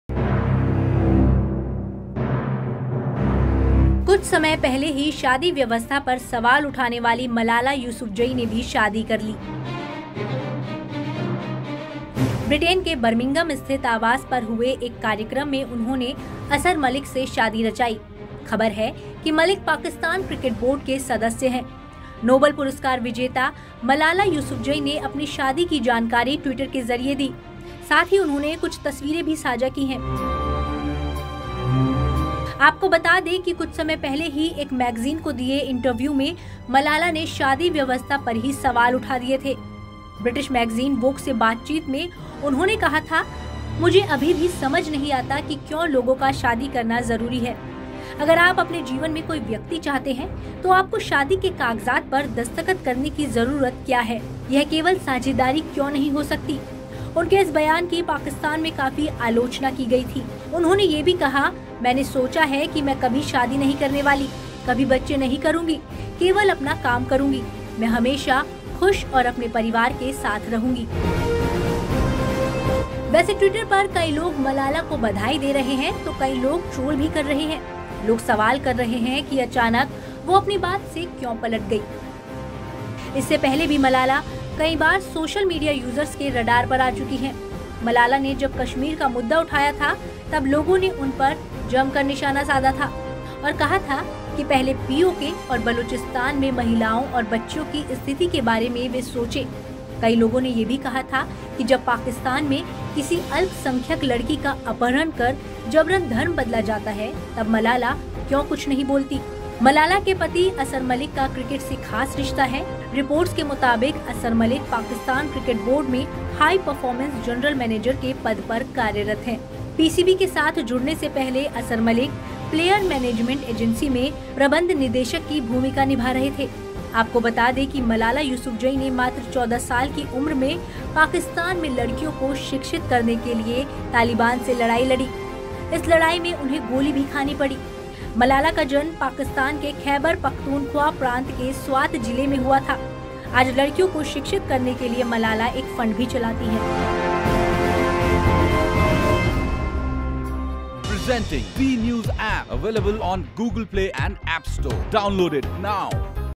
शादी करके बुरा फंस गईं मलाला ! | Malala Yousafzai Marriage | Hindi News
सबसे कम उम्र में नोबल पुरस्कार से सम्मानित होने वालीं मलाला यूसुफजई ने शादी रचा ली है. उन्होंने ब्रिटेन के बर्मिंघम स्थित अपने घर पर परिवार की मौजूदगी में असर मलिक के साथ निकाह किया. मलाला ने सोशल मीडिया पर अपनी शादी की तस्वीरें शेयर की हैं.